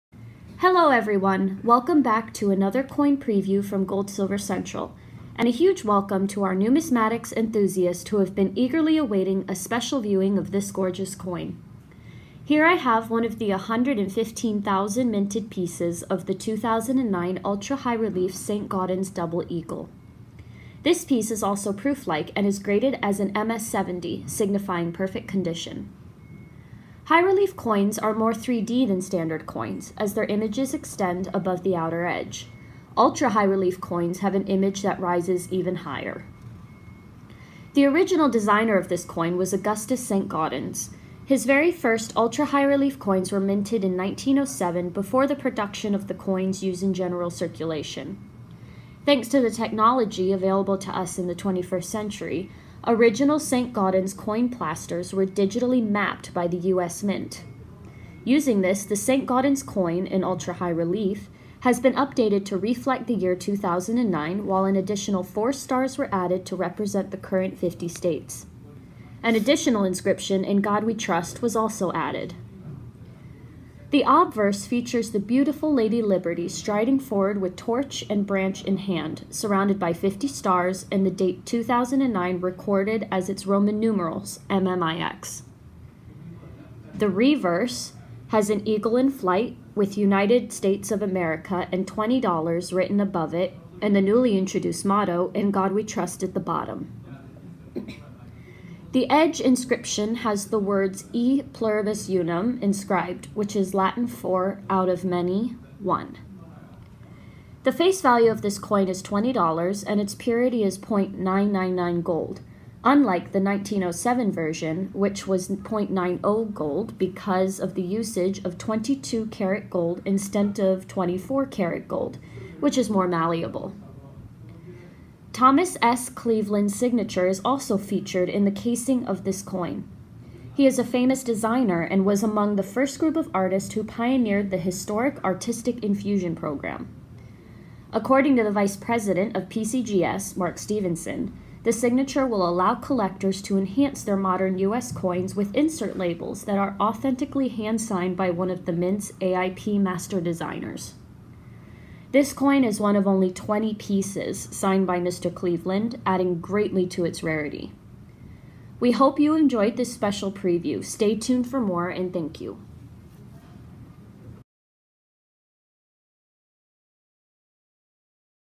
Only 20 pieces: 2009 Ultra High Relief St. Gaudens MS-70PL Gold Coin (Thomas Cleveland's Signature)
Here, we have one of the 115,000 minted pieces of the 2009 Ultra High Relief St. Gaudens Double Eagle. This piece is also proof like and is graded as an MS70, signifying perfect condition.

This piece is also extremely rare with only 20 pieces available with Mr Thomas S Cleveland's Signature on the slab.